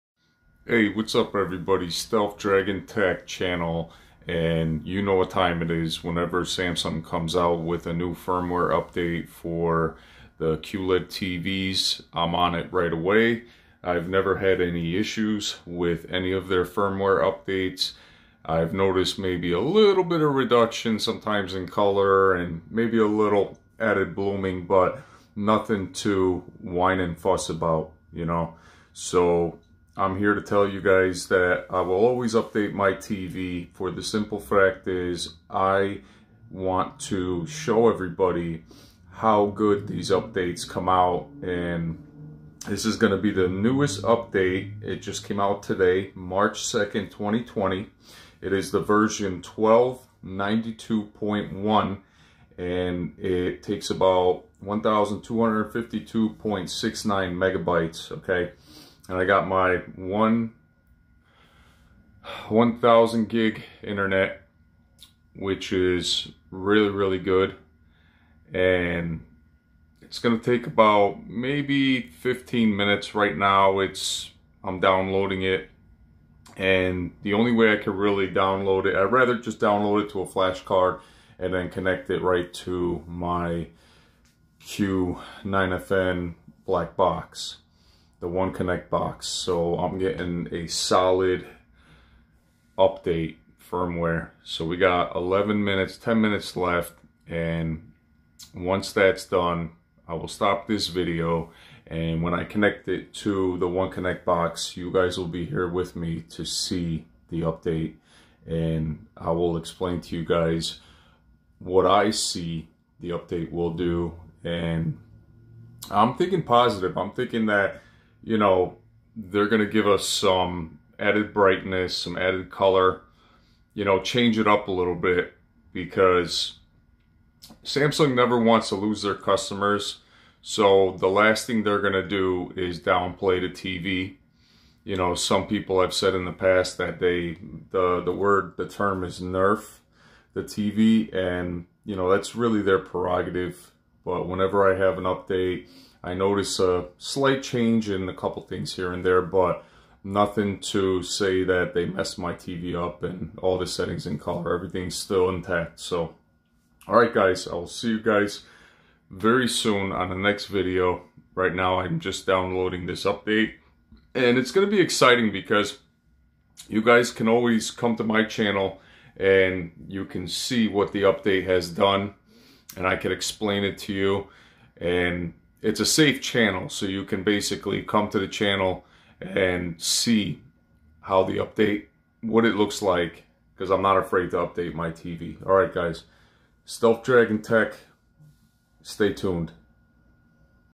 Samsung firmware 1292.1 latest version!
Getting ready to add the new firmware 1292.1 to the Samsung q9fn!